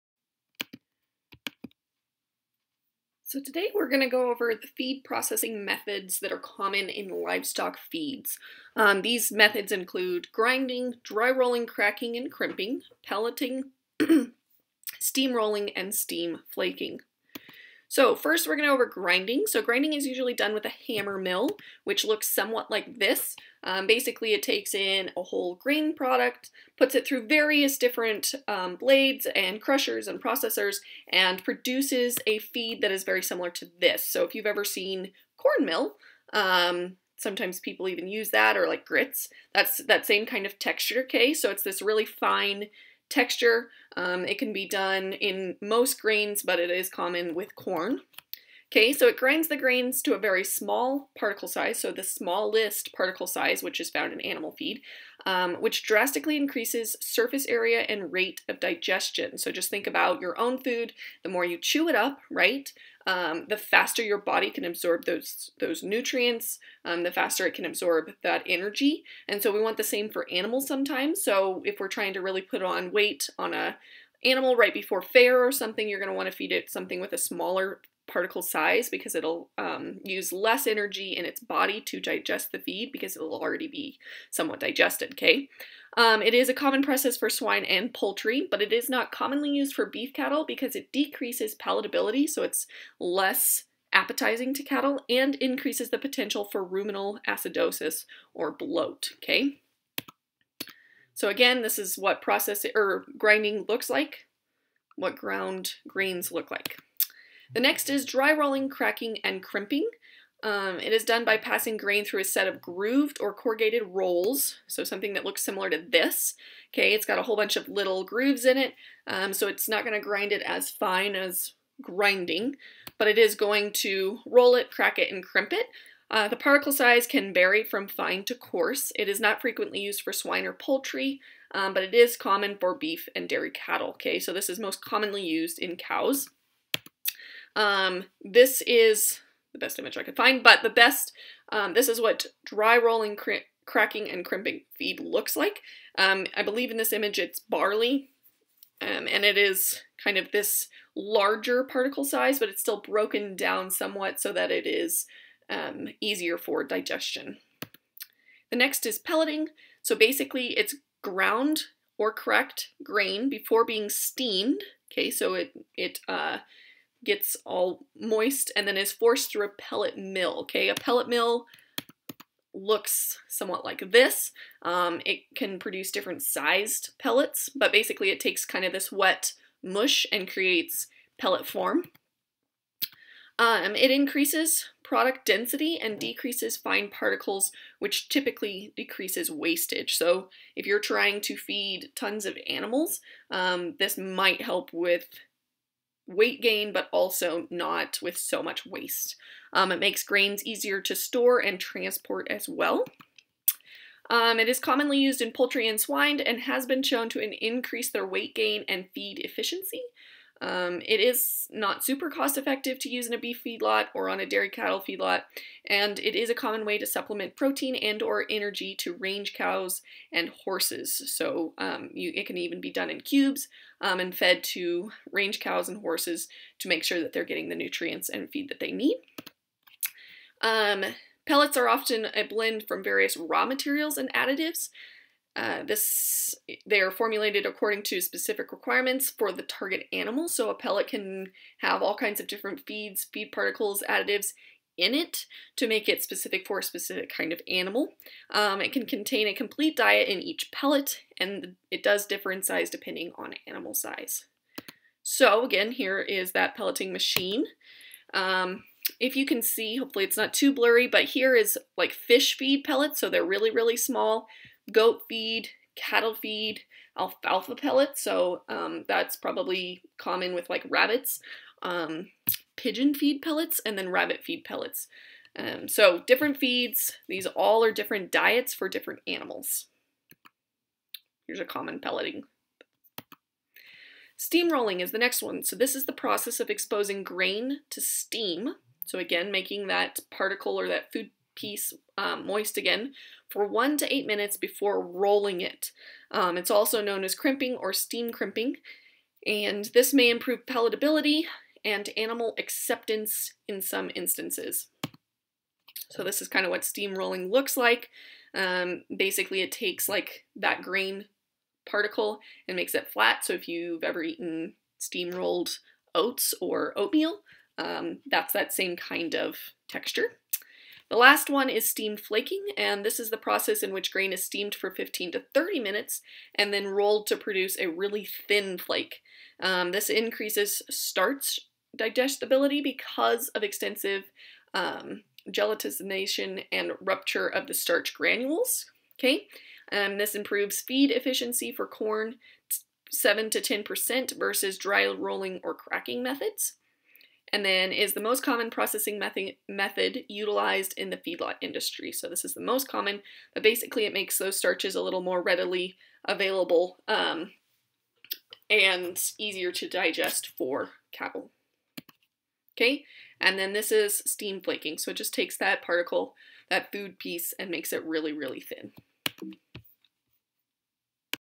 Feed Processing Methods
Video goes over basic feed processing methods found in the livestock industry.